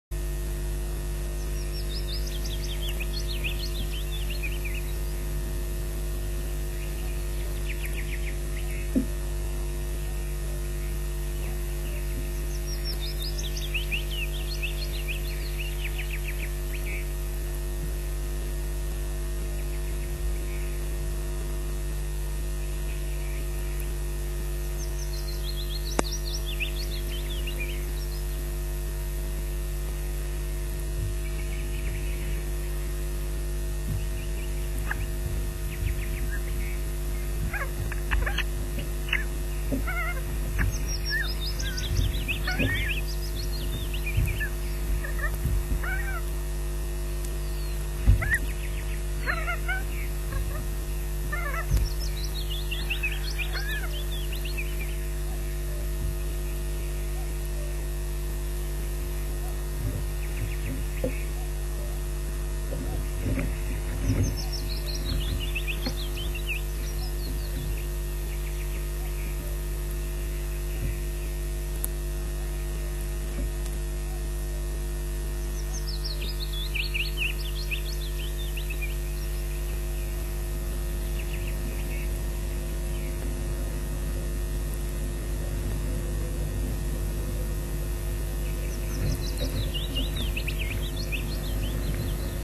Loch Garten Ospreys 2011 3rd June 10.16 - fighting fit
After a lazy time together, the two growing chicks become active again. A fight ensues.